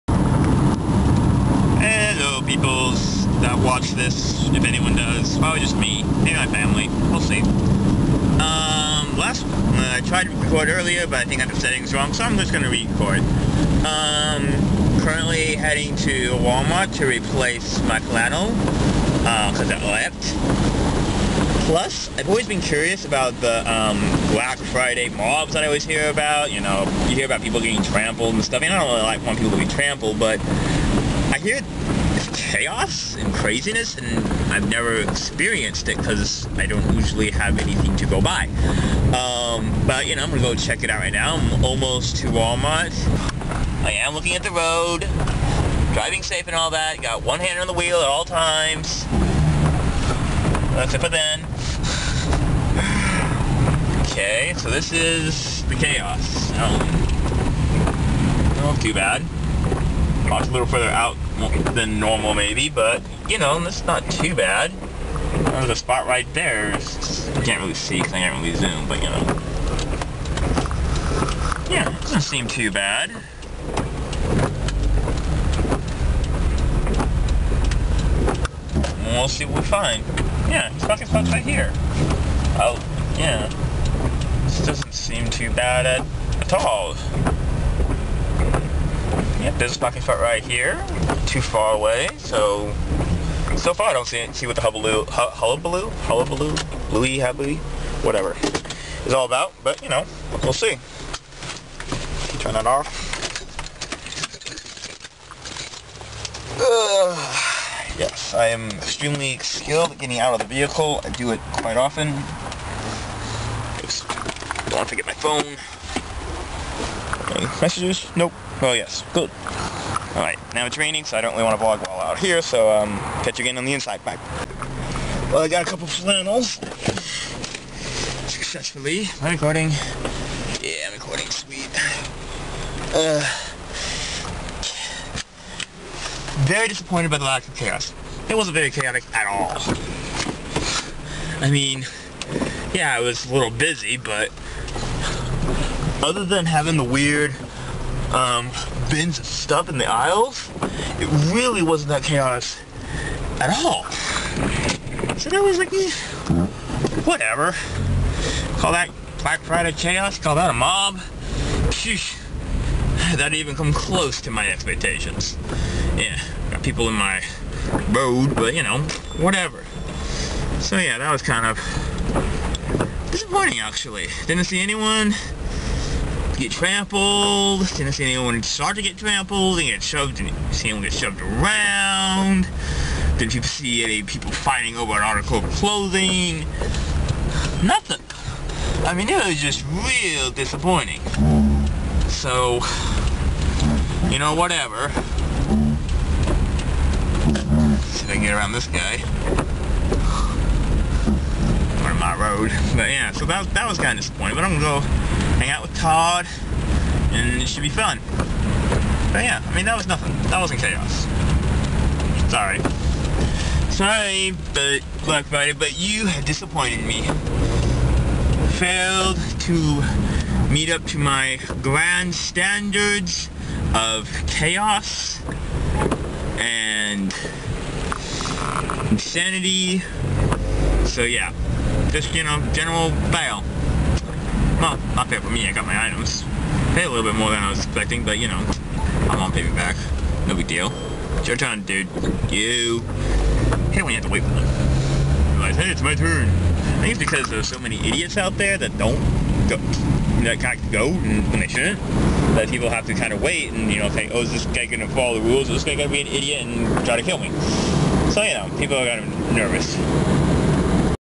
Orange Hat's Vlog #2
My second vlog! Probably less entertaining cuz it's just me talking.